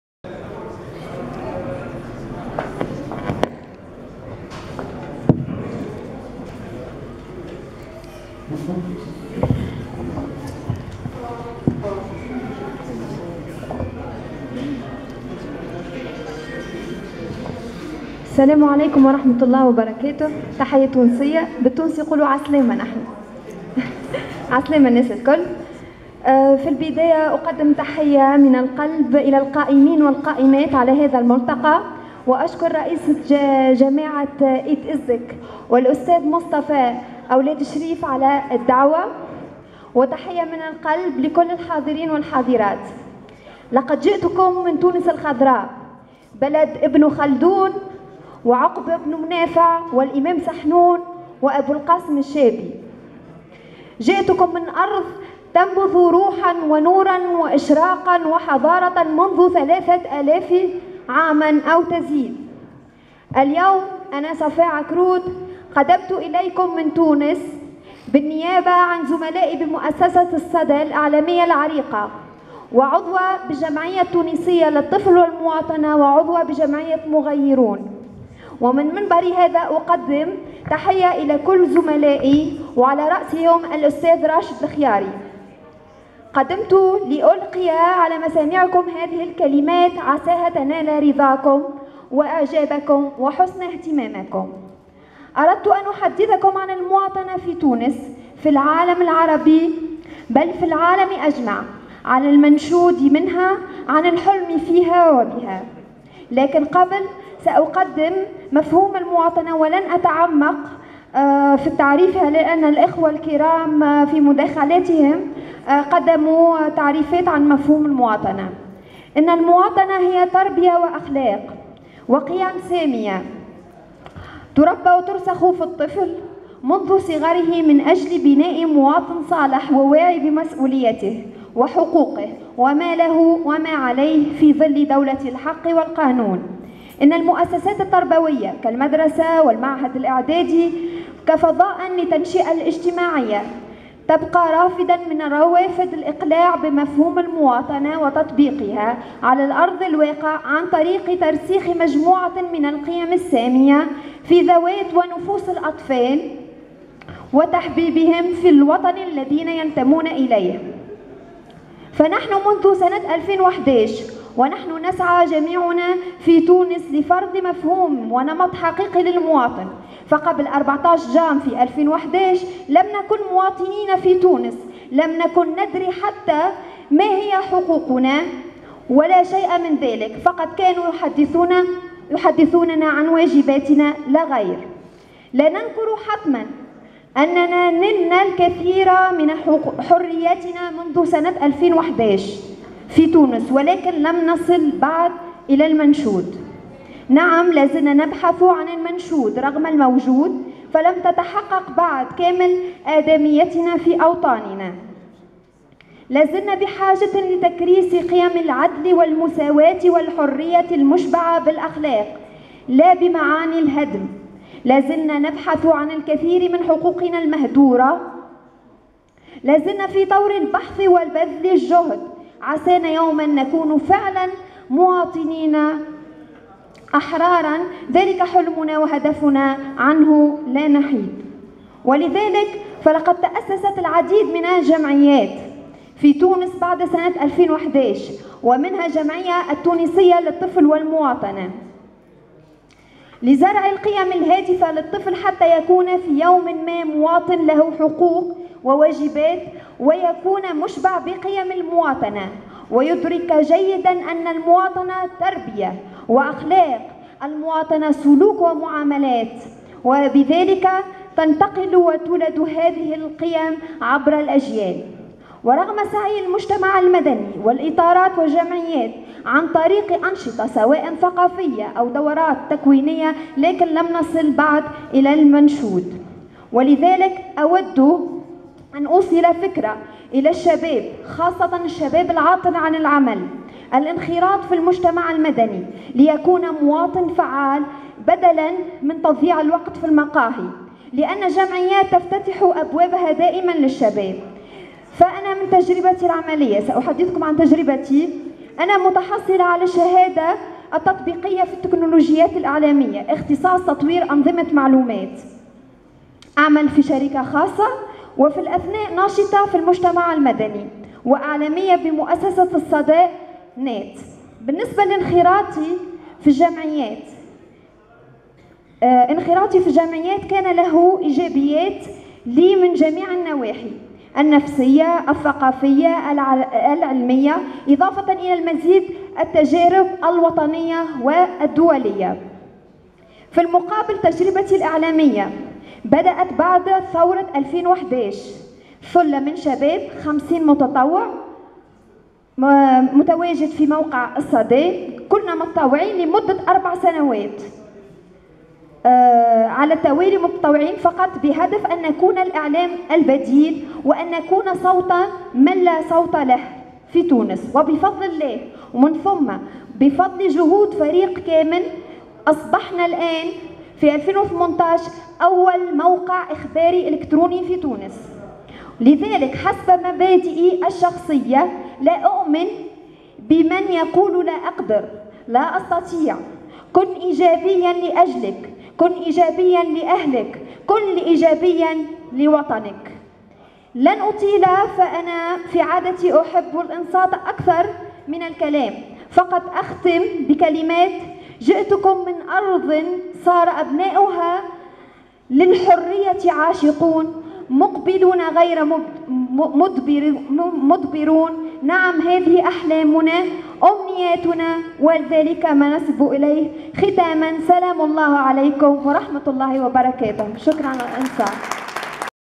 مداخلة "موفدة موقع الصدى" في الملتقى الدولي للتنمية المحلية بالمغرب
مداخلة "موفدة موقع الصدى صفاء عكروت" في الملتقى الدولي للتنمية المحلية بالمغرب تحت عنوان "المواطنة بين الخصوصية المحلية و العالمية